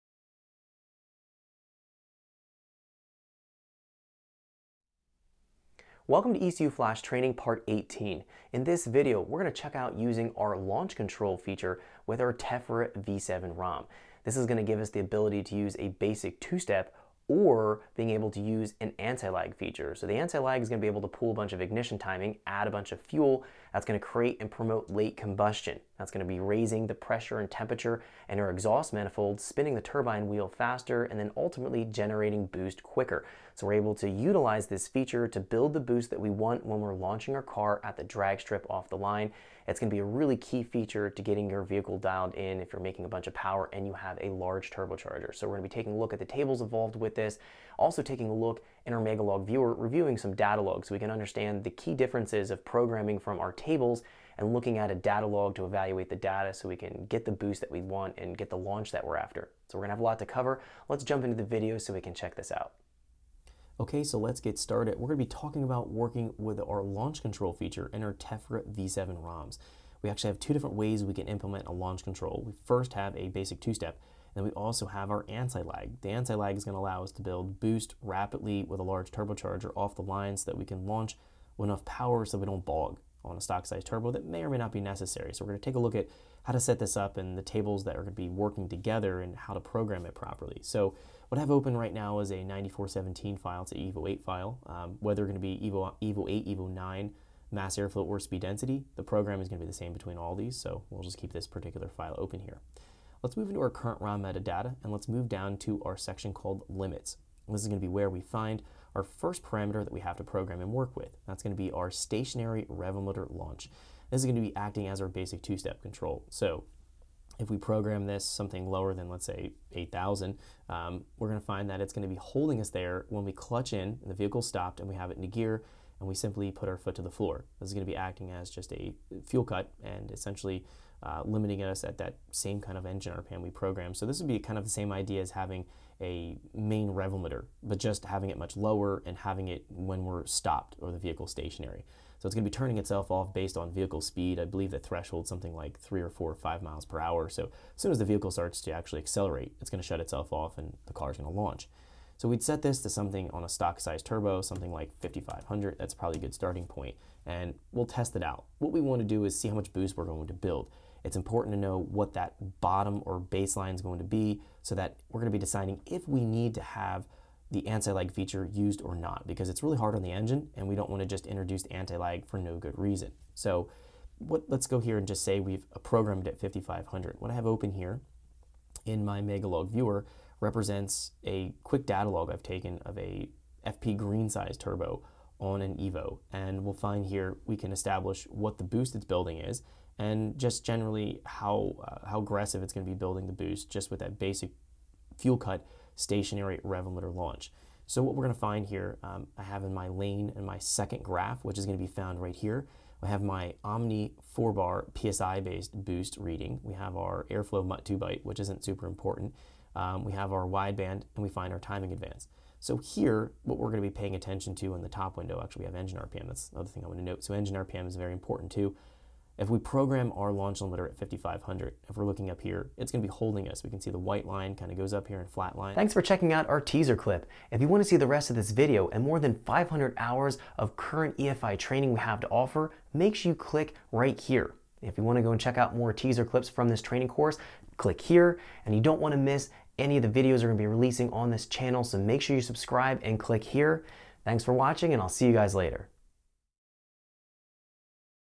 Ecu Flash Training Course Part 18: Anti-Lag Launch Control | Evans Performance Academy
Ecu Flash Training Course Part 18: Anti-Lag Launch Control walks you through how to properly program the basic launch control / 2-step feature as well as anti-lag to build boost on a big turbo.  We explore how to analyze data logs in Megalog viewer to determine what changes to make in the launch control tables to achieve your desired results.